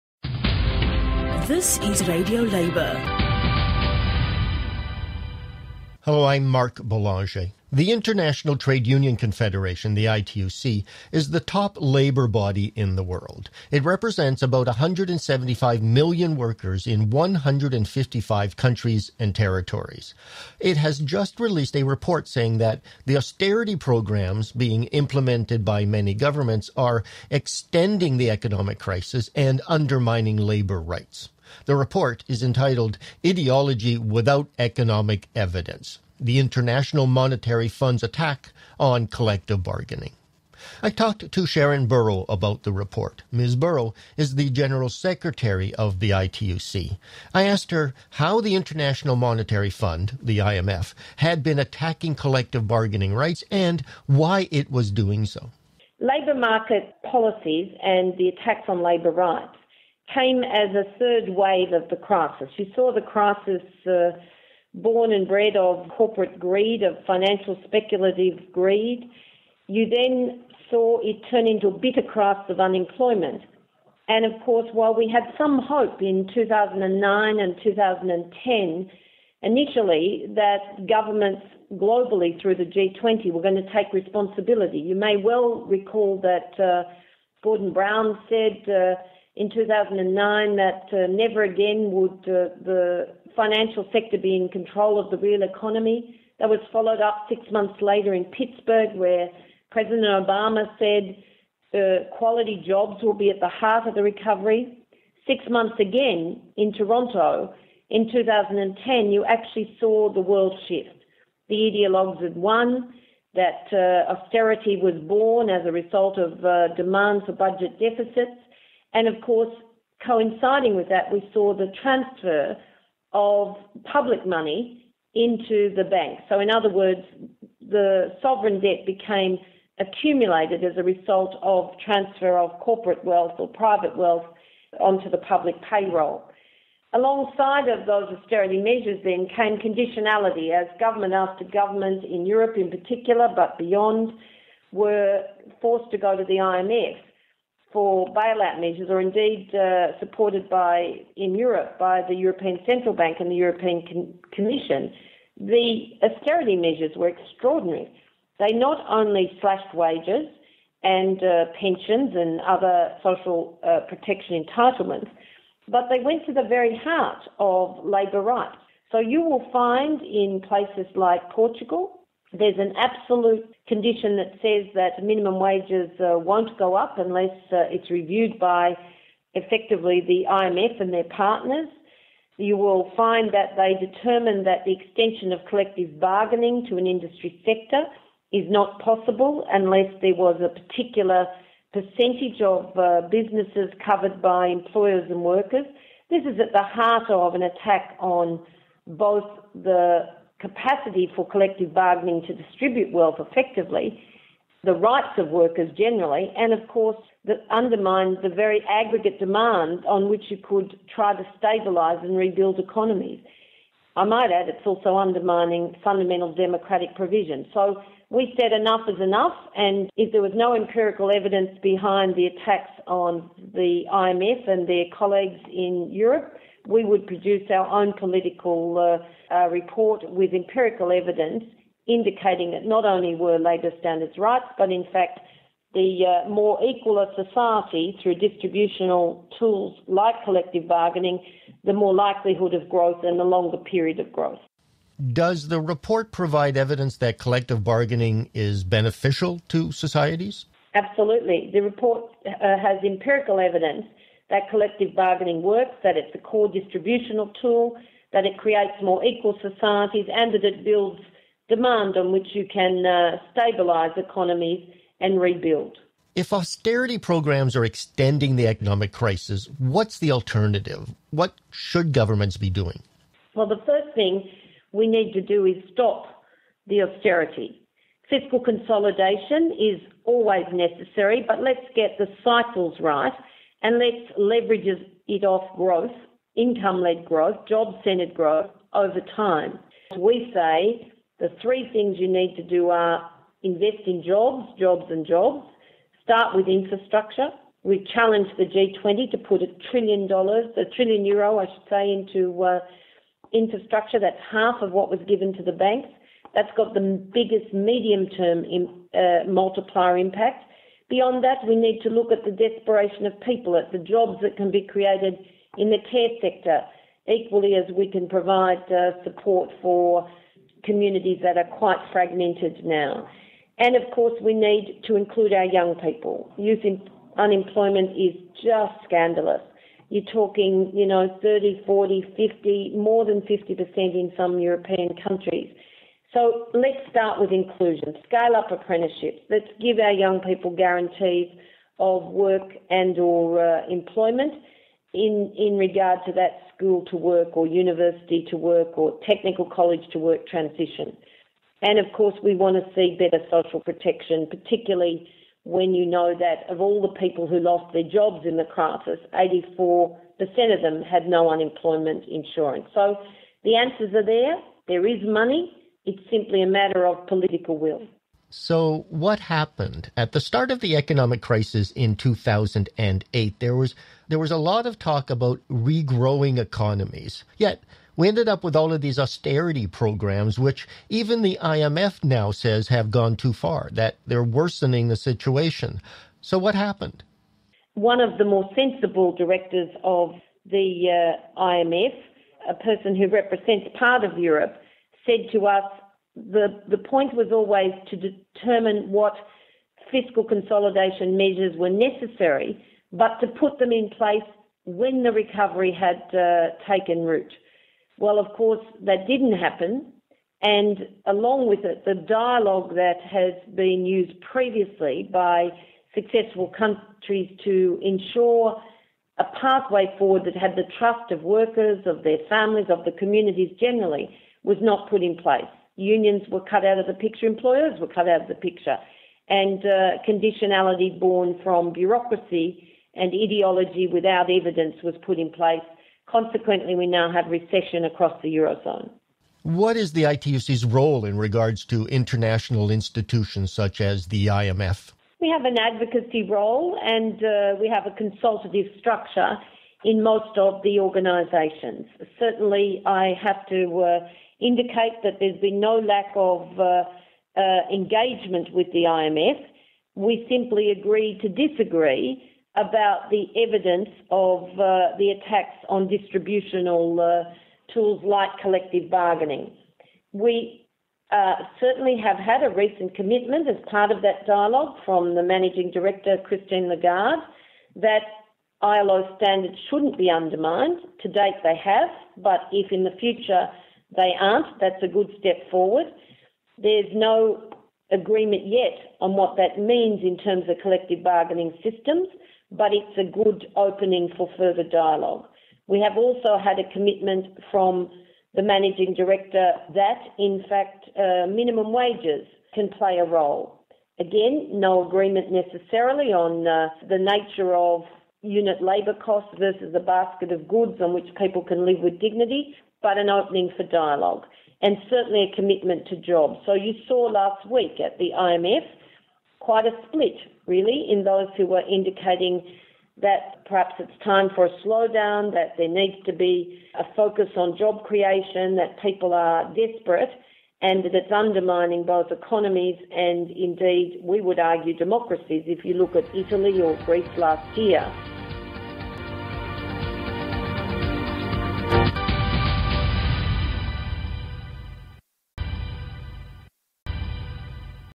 Sharan Burrow
The attack on collective bargaining by the International Monetary Fund